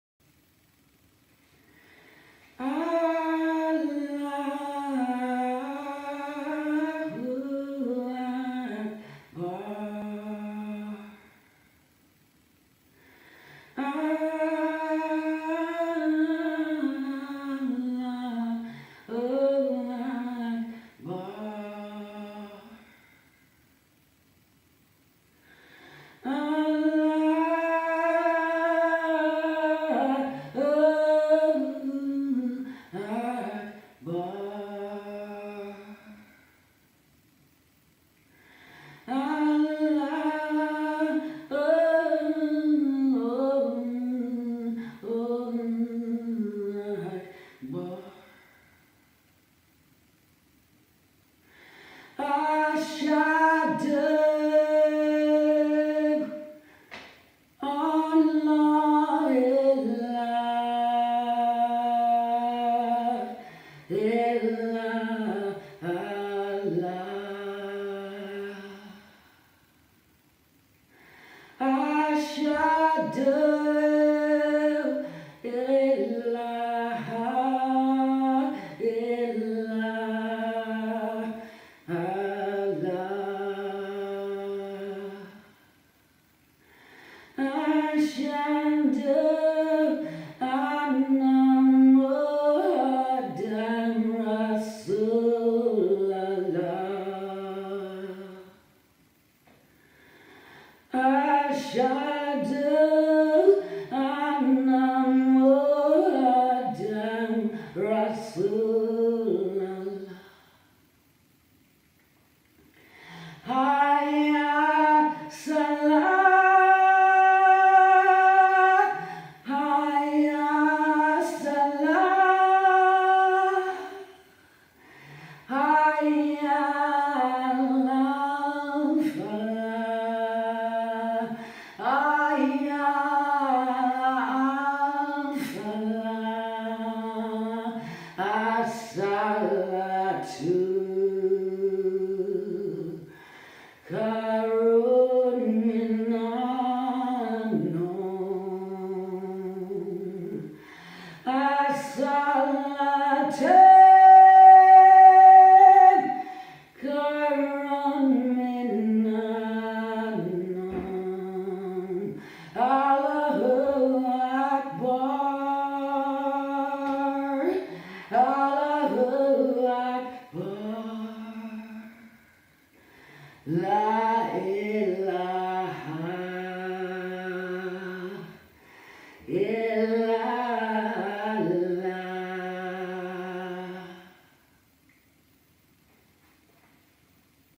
Sinead O'Connor Converts To Islam"Muslim" Video.
Sinéad O'Connor Explains Why She Left Christianity and Accepts Isam.